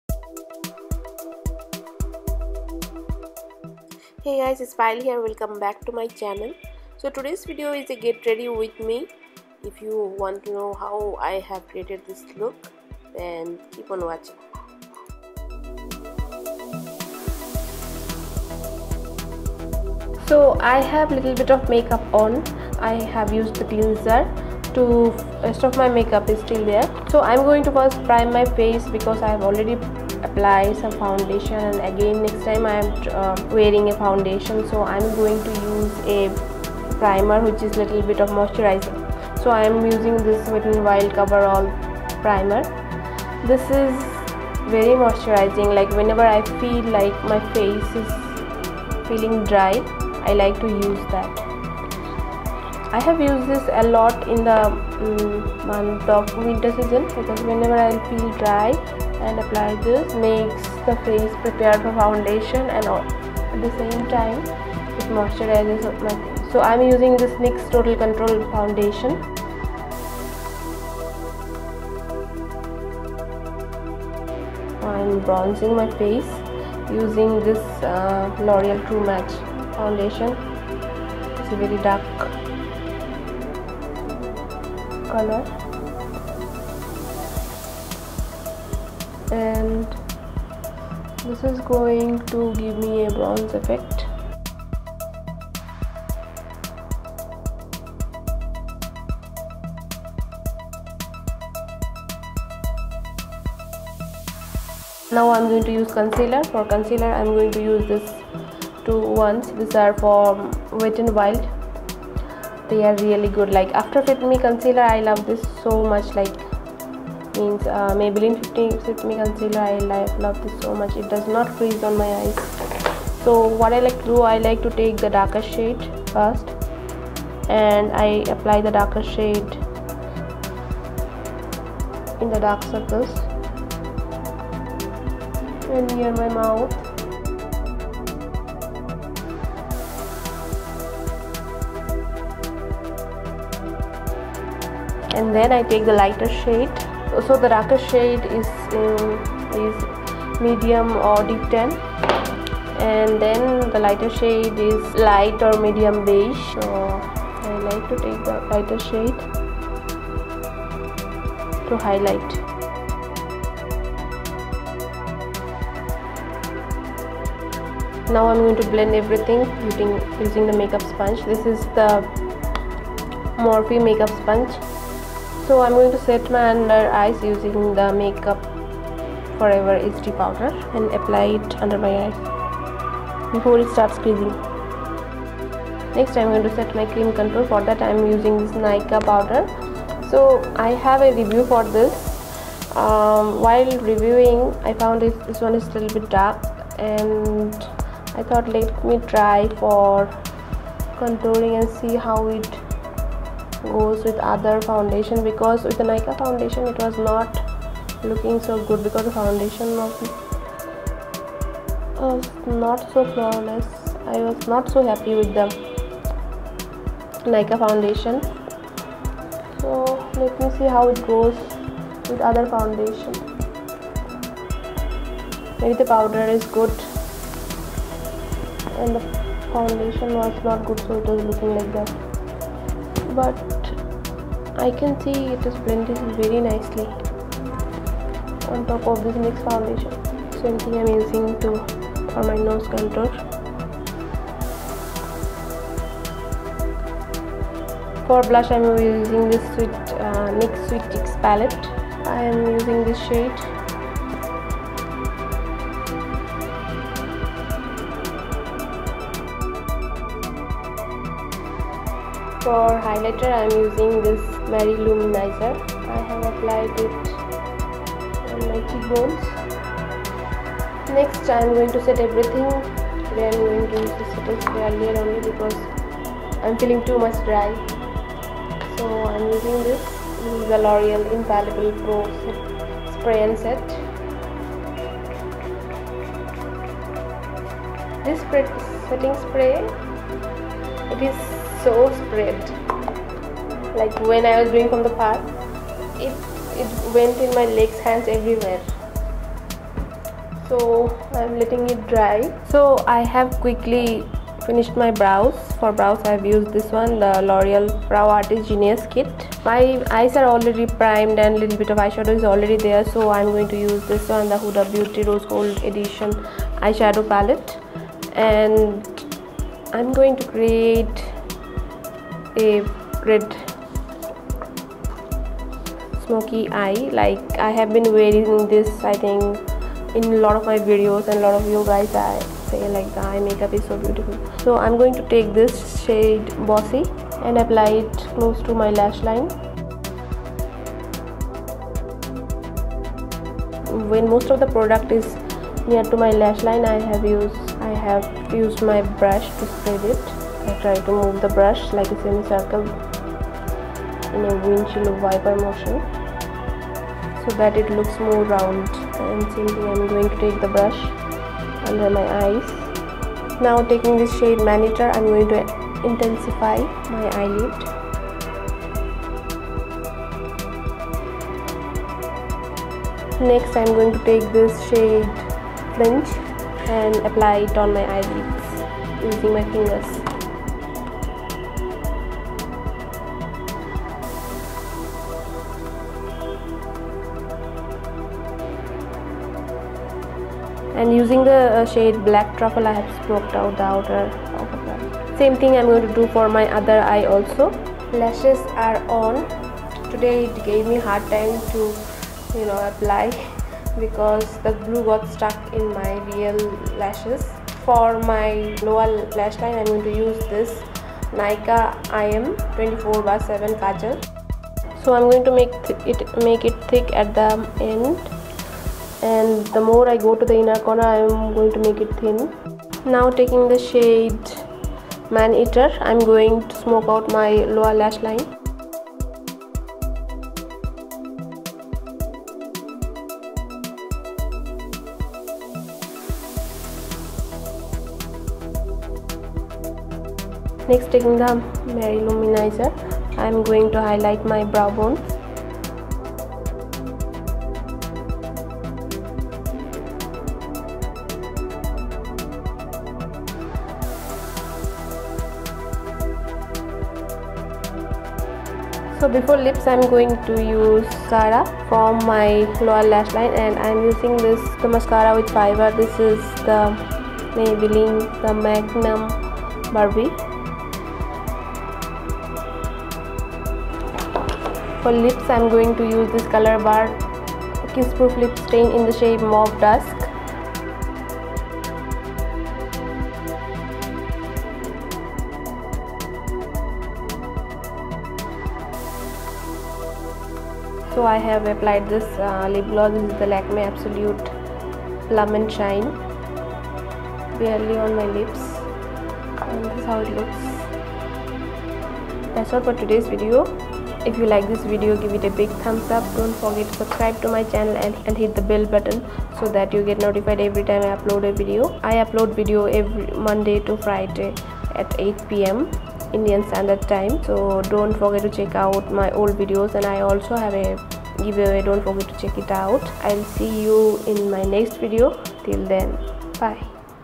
MAKEUP TRANSFORMATION | Get Ready With Me | Extreme Glam
Hi Guys, it’s Payel here.

Products Used:
Wet n Wild Cover All Face Primer - Partners In Prime

Makeup Forever HD Setting Powder

Huda Beauty Faux Mink Lash - Noelle #14